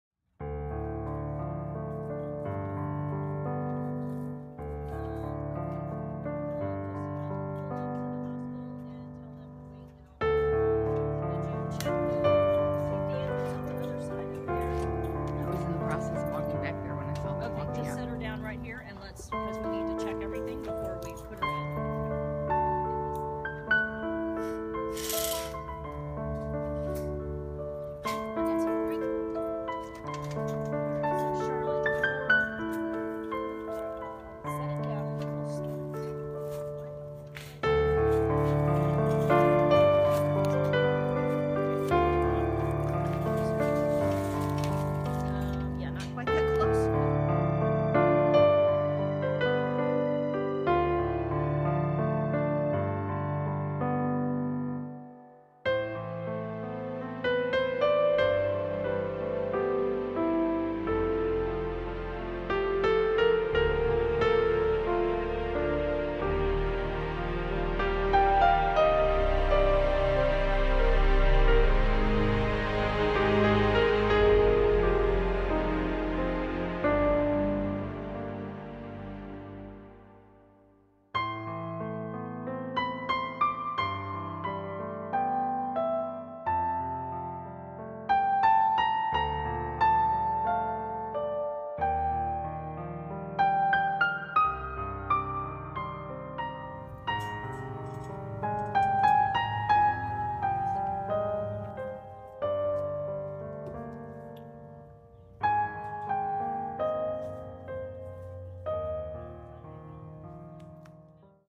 Kalahari Serval Goes Home
Kalahari Serval Goes Home after having a mass removed and recovering in the Cat Hospital.